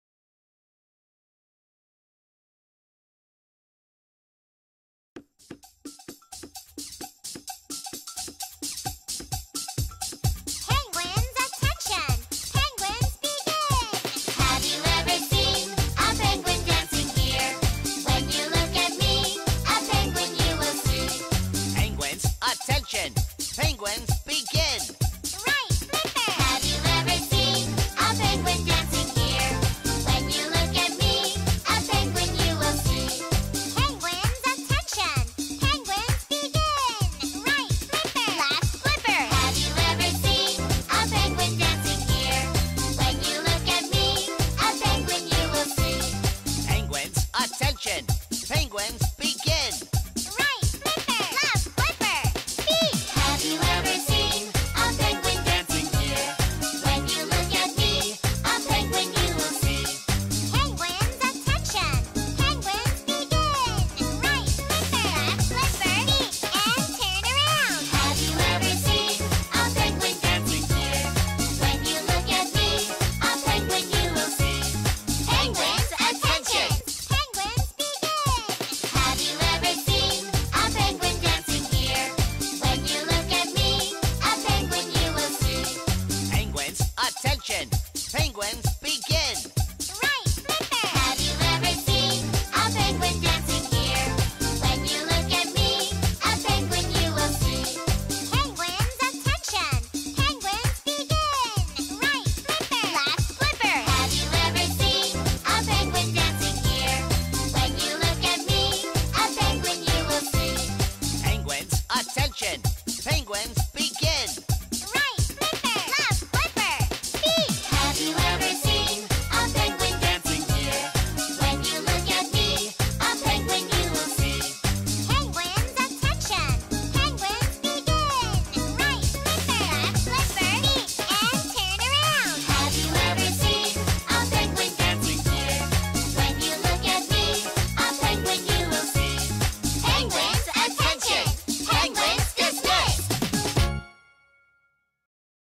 Playschool - Penguin
Kangaroo Kids | Billabong High International Fuvahmulah
Annual Concert 2017" Skippy's Underwater Adventure"  Show 2